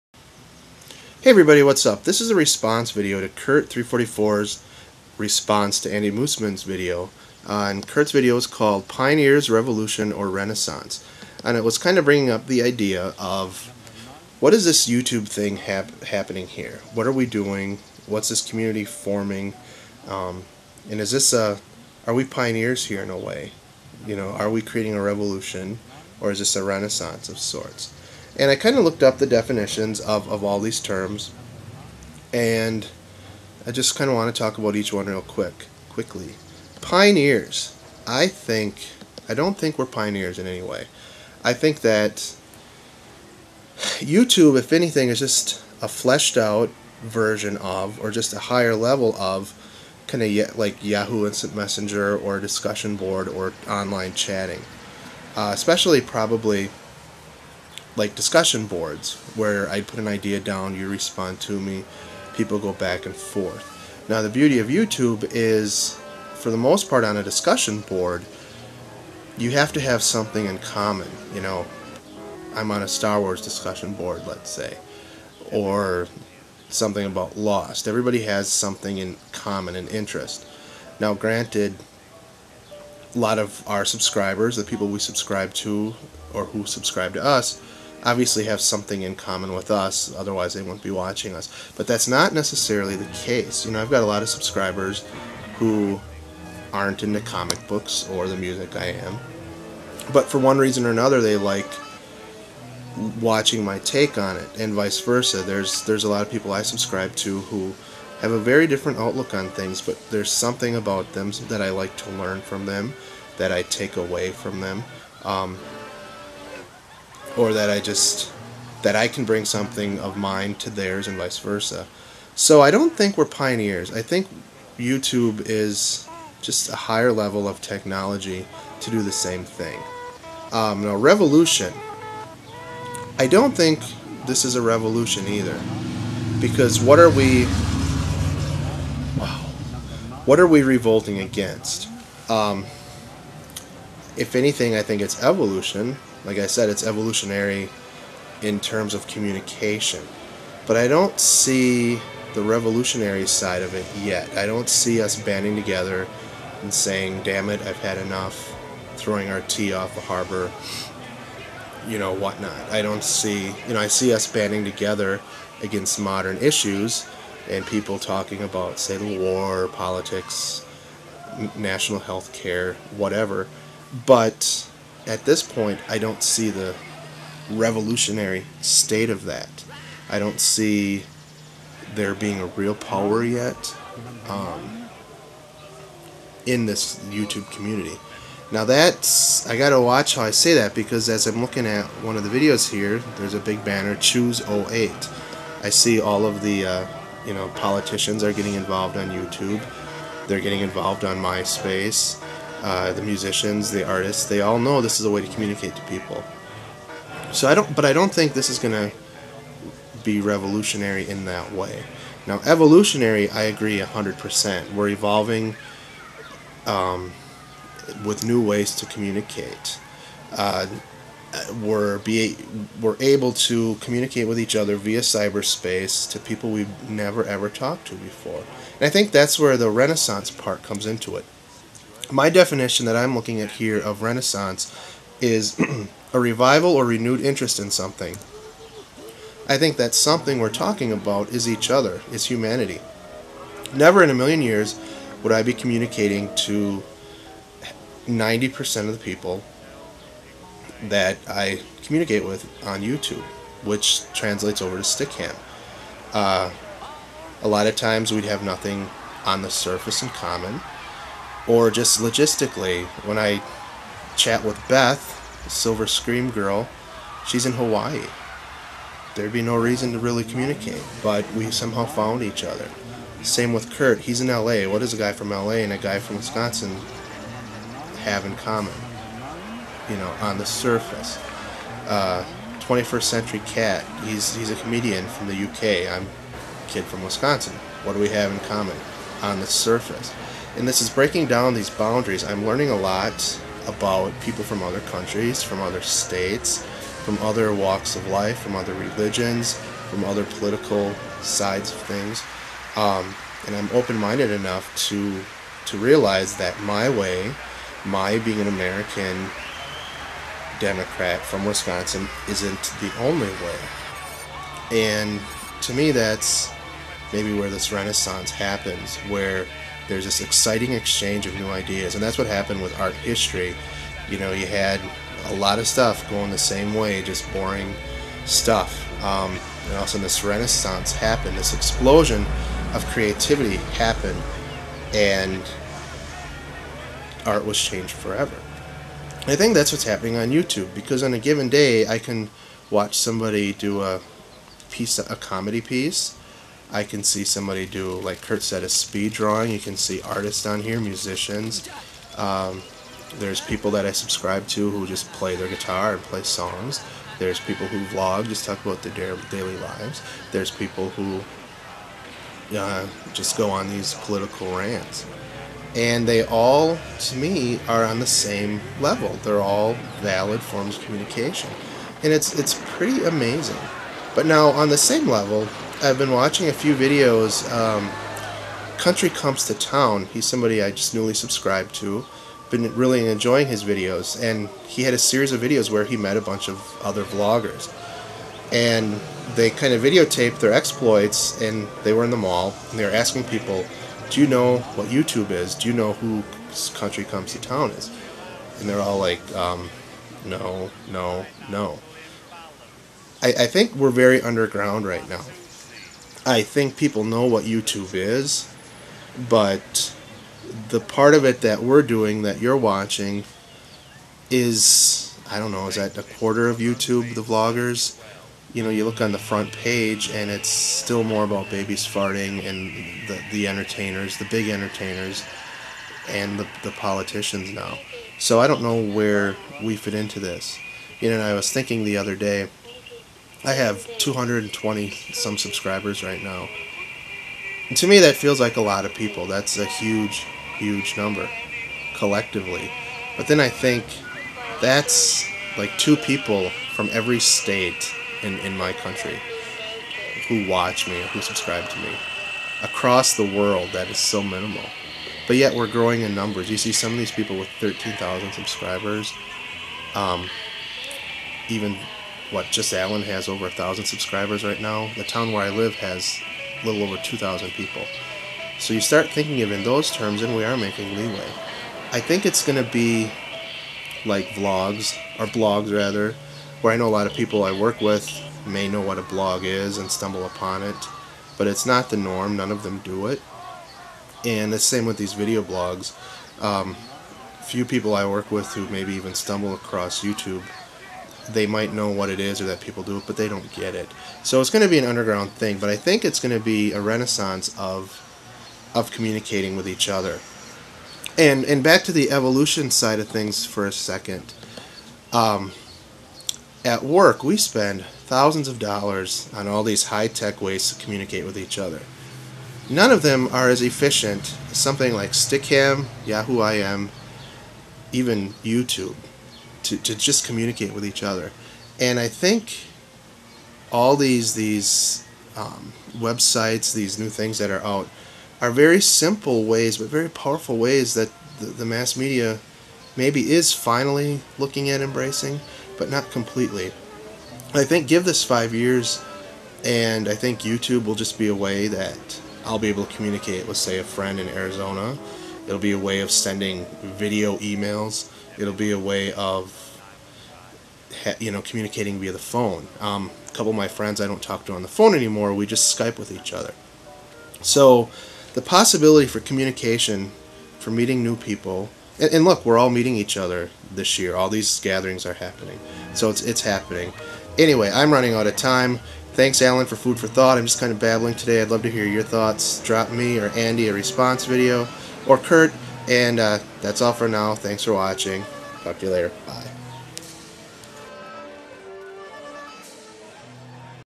Re: Pioneers? Revolution? Renaissance?
Here are my thoughts on the idea of our YouTube community. Are we pioneers? Is a revolution happening? Is this a renaissance?

These people all rock my vloggerhood.